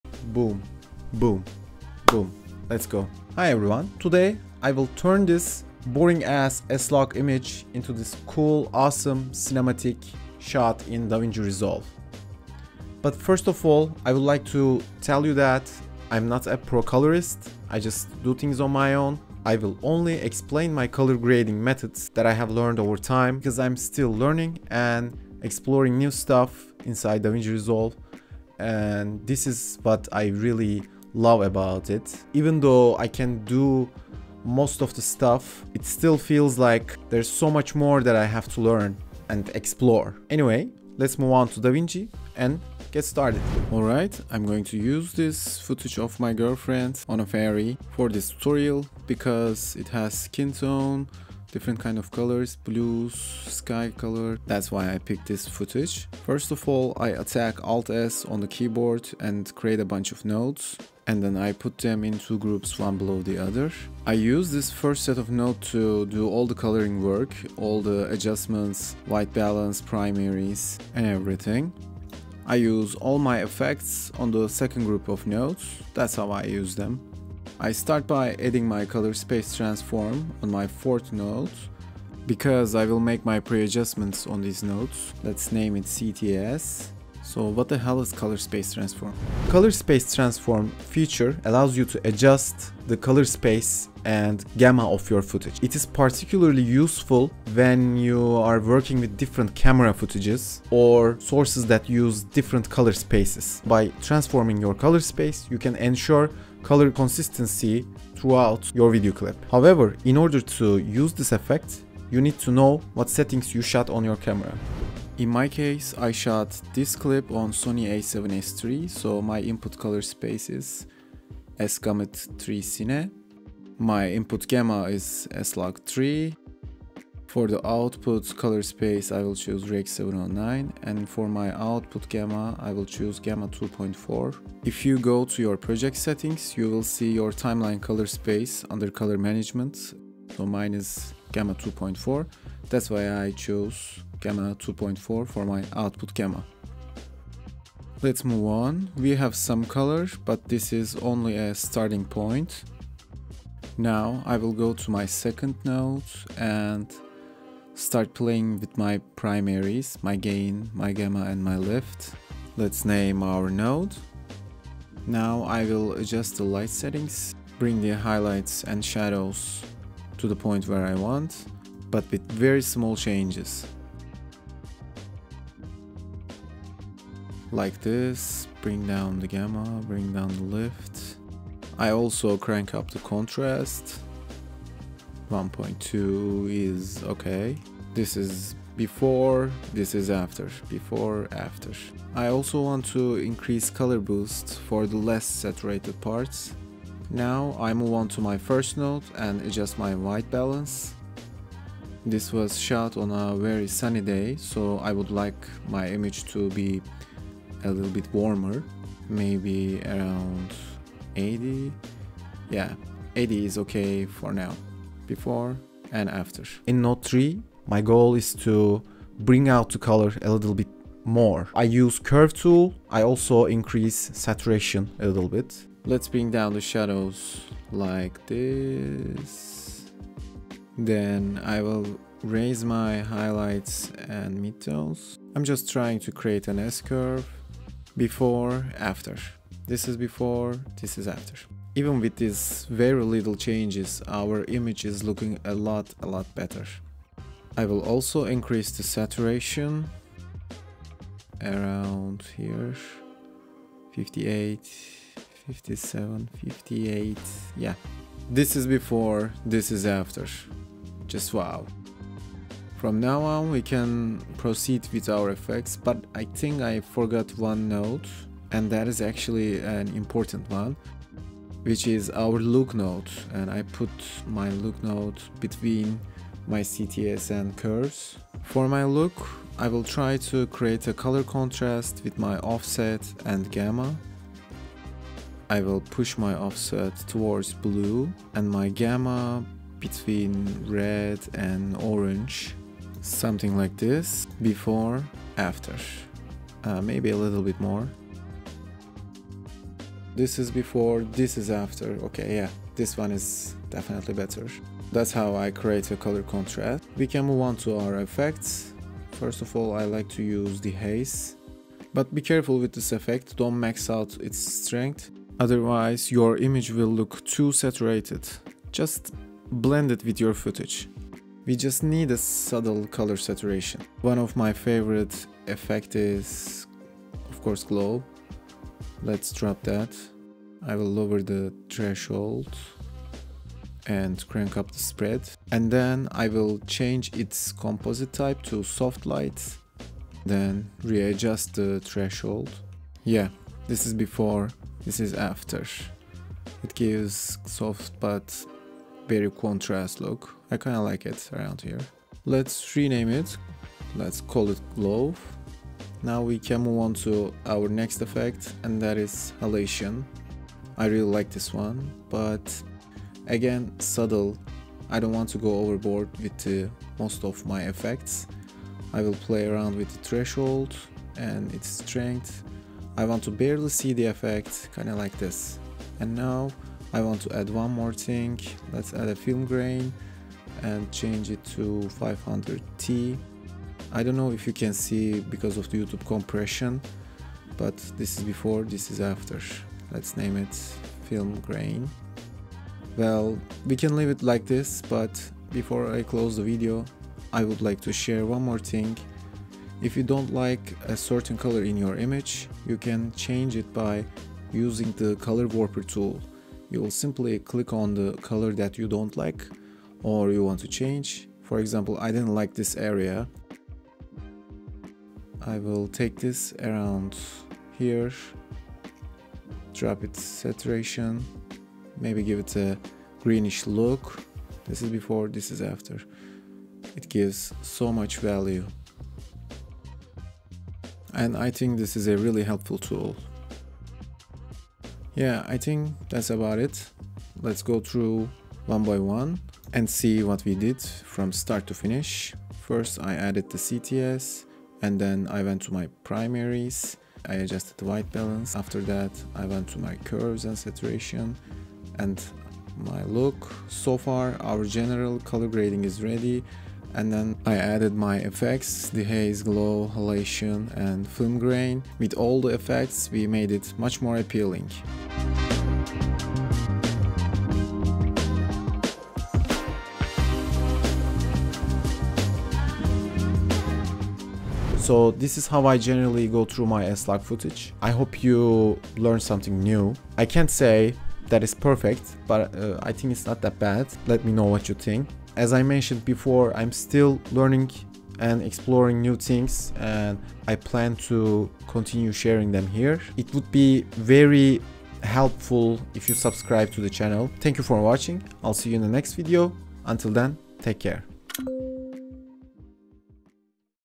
My COLOR GRADING Process in Davinci Resolve 18 | Step by Step Tutorial
in this video I tried to explain my own color grading process in Davinci Resolve. I hope it was effective.  If you have any thoughts or comments, please let me know.

You can check out my cinematic LUT pack on my Etsy store

Gear I use :
(Wireless Microphone)
( ND Filter)